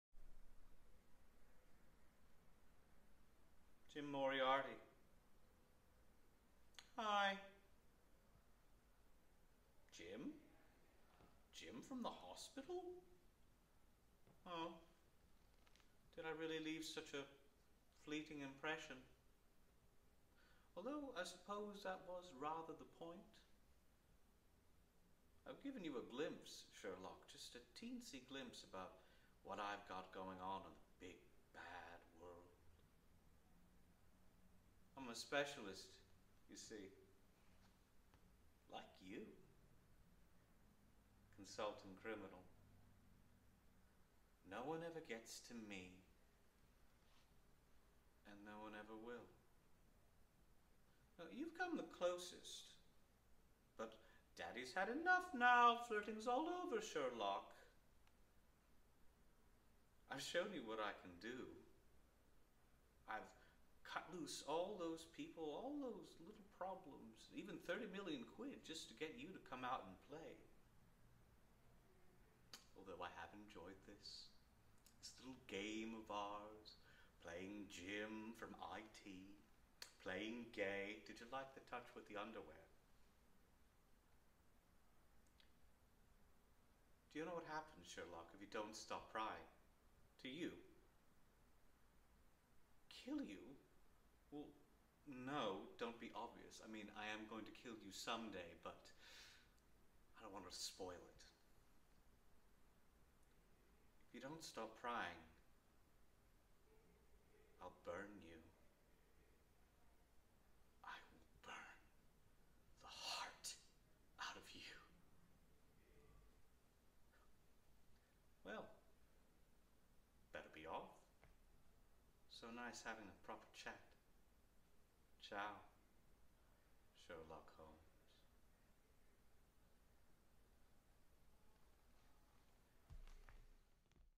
John Galas - Moriarty Monologue
A video of actor John Galas performing a monologue from "Sherlock" in the role of Jim Moriarty.

Check out what else I'm up to